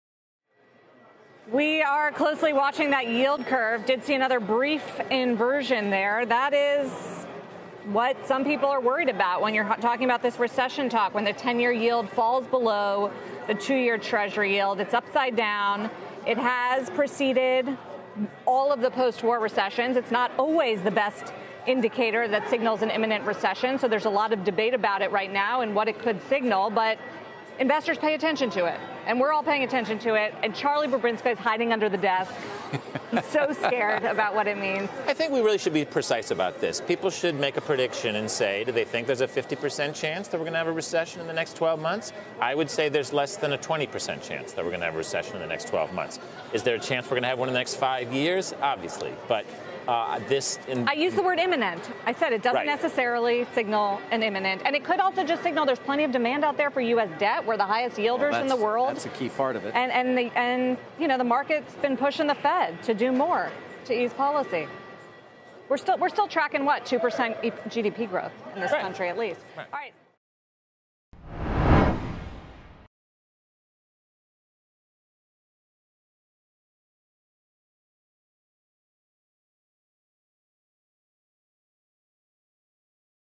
Yield curve briefly inverts for the first time since August 15
The yield curve briefly inverts again for the first time since August 15, with the 2-year Treasury yield topping the 10-year rate.